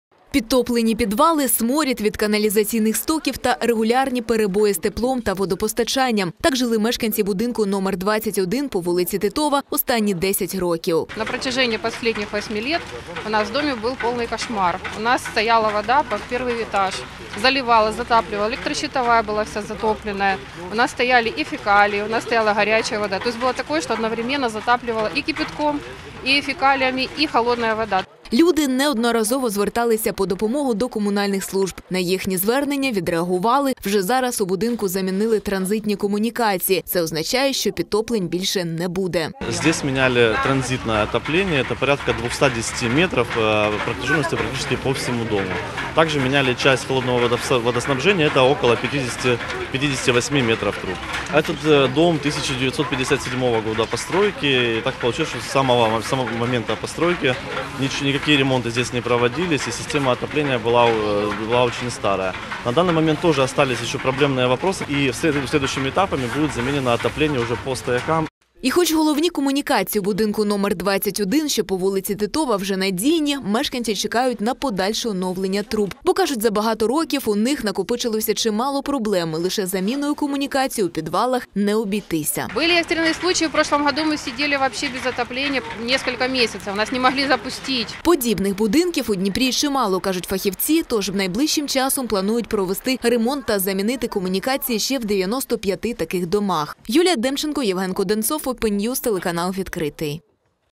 У багатоповерхівці на вулиці Титова проклали нові комунікації
Мешканці будинку №21 по вулиці Титова отримали нові труби транзитного опалення та водовідведення. І тепер можуть зітхнути з полегшенням - підтоплень більше не буде.